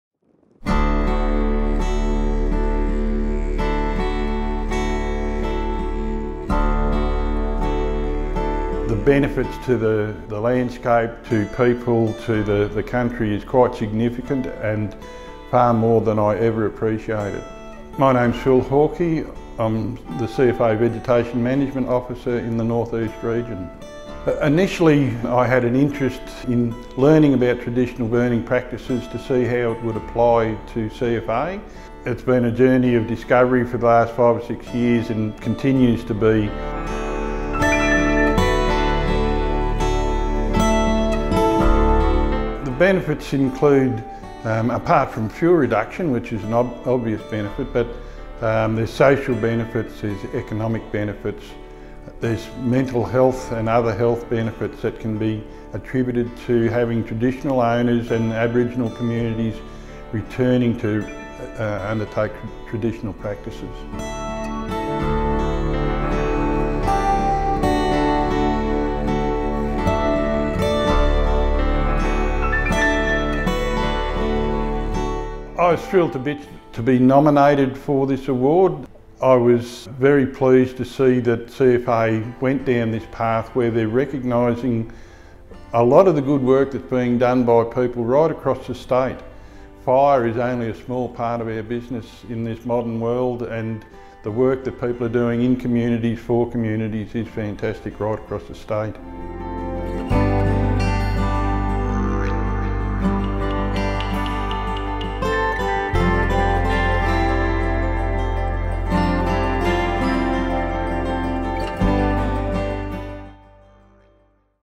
Traditional burning brings many benefits
To mark National Reconciliation Week 2018 we speak to CFA's Vegetation Management Officer for the North East, Phil Hawkey, who has spent the past 5 years working with Aboriginal communities to learn more about traditional burning practices.